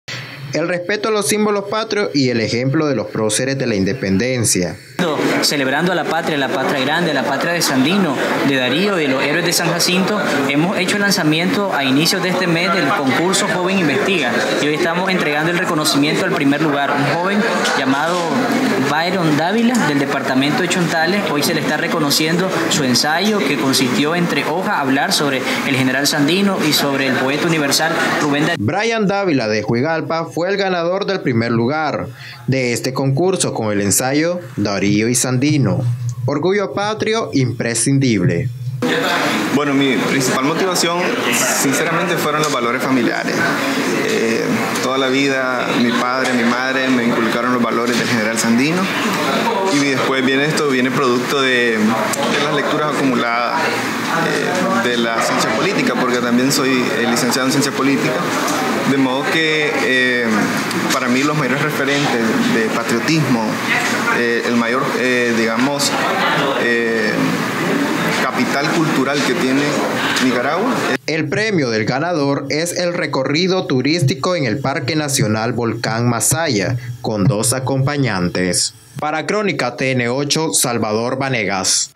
MINJUVE premia al ganador del concurso "Joven Investiga" – Nicaragua
El Ministerio de la Juventud realizó este jueves la premiación del concurso virtual "Joven Investiga", donde se destacó el orgullo patrio de los nicaragüenses.

Es así que el amor a la patria se plasmó desde la cultura e historia, el respeto a los símbolos patrios y el ejemplo de los próceres de la independencia.

"Efectivamente el Ministerio de la Juventud conmemorando a la patria grande, a la patria de Sandino, hicimos el lanzamiento a inicio de este mes del concurso: 'Joven Investiga' y hoy estamos entregando el reconocimiento al primer lugar, un joven llamado Bryan Dávila del departamento de Chontales. Hoy se la está reconociendo su ensayo que consistió en tres hojas, habló sobre el General Sandino y sobre el poeta universal, Rubén Darío, y esta es una muestra más de la motivación, inspiración que tiene nuestra juventud nicaragüense, para seguir escribiendo la historia, para seguir redescubriendo todos los elementos importantes que nos permite ir hacia adelante", expresó Milton Ruiz, Coordinador de la Juventud Sandinista.

"Así que el Ministerio de la Juventud impulsa estrategias que nos permiten desarrollar la educación, la investigación, y por supuesto el conocimiento de nuestra historia que es una historia viva, una historia que nos cobija a todos y que está marcada por la lucha de nuestros pueblos todos los días", agregó Ruiz.

Ejemplo de dedicación
Bryan Dávila, de Juigalpa, fue el ganador del primer lugar  de este concurso con el ensayo: "Darío y Sandino, Orgullo patrio imprescindibles".

"Mi principal motivación fueron los valores familiares, toda la vida mi padre, mi madre me inculcaron los valores del General Augusto C. Sandino y también viene producto de las lecturas acumuladas de las ciencias políticas, de modo que para mí los mayores referentes de patriotismo, el mayor capital cultural que tiene Nicaragua, es el poeta universal, Rubén Darío y el máximo héroe que nos defendió con la espada, el General Sandino", dijo el joven.

"Entonces son para mí mis mayores referentes de patriotismo, como ya se decía anteriormente en la encuesta M&R precisamente, decían que Sandino y Rubén Darío son reconocidos por los nicaragüenses como los más altos referentes de patriotismo de Nicaragua", agregó.

El premio del ganador es el recorrido turístico en el Parque Nacional Volcán Masaya con dos acompañantes.

Jóvenes de 10 municipios participaron en esta actividad cultural.

Noticiero Crónica emitido el 24 de septiembre 2020